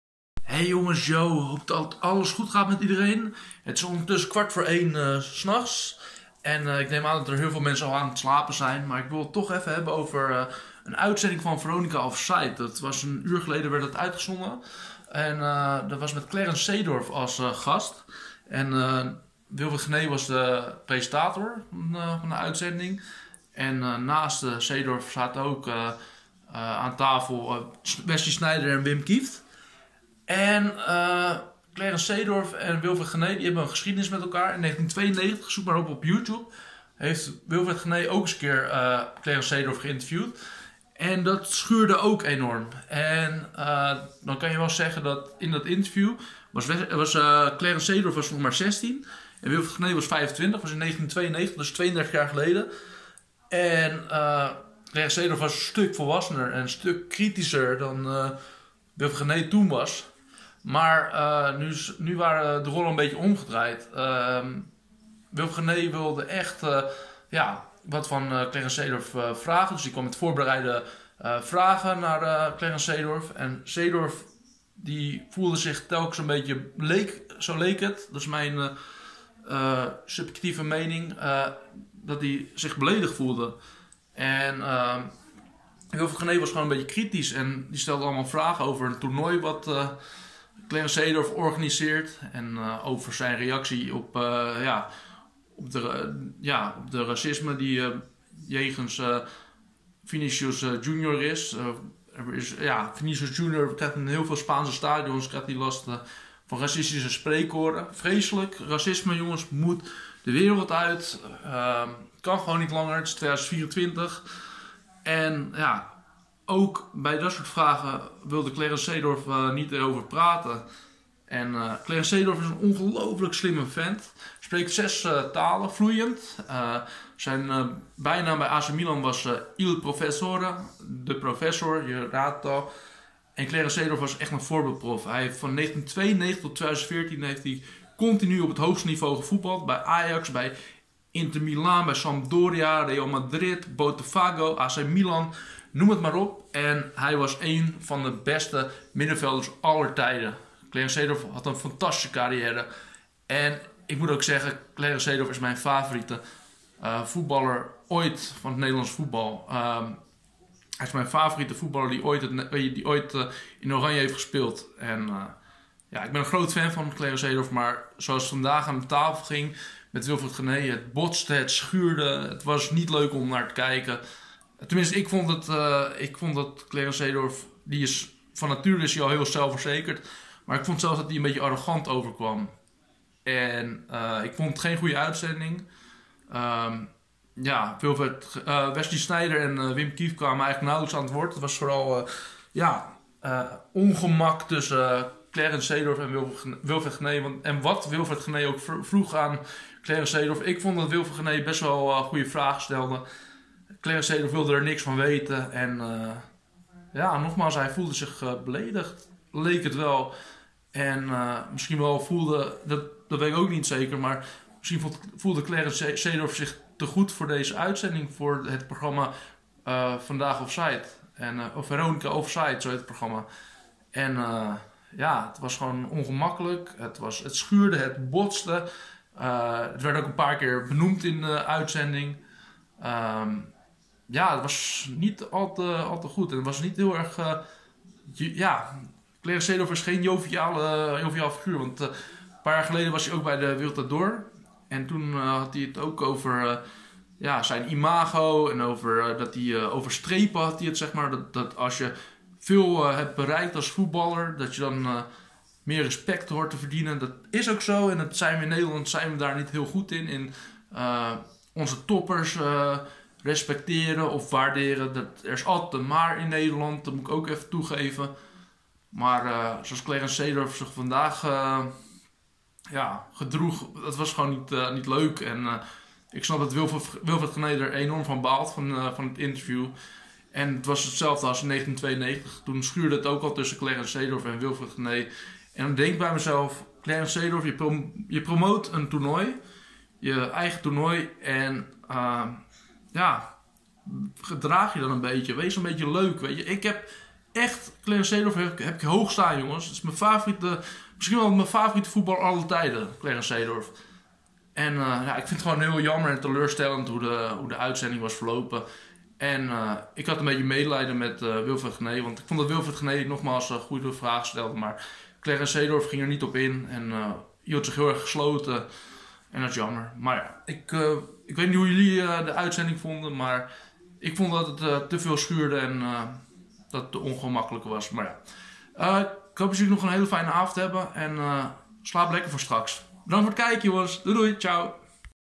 Even praten over het optreden van het fenomeen Clarence Seedorf bij Veronica Offside 25-03-2024
Begrijp me niet verkeerd ik ben echt een fan van de voetballer en mens Clarence Seedorf. Hij is ontzettend slim en was altijd een voorbeeldprof. Ik had veel verwacht van deze uitzending, want ik heb Clarence Seedorf hoog staan. Helaas botste het enorm tussen Wilfred Genee en Clarence Seedorf. Volgende keer hopelijk weer een leuker interview met Clarence Seedorf. Blijf het een geniale middenvelder vinden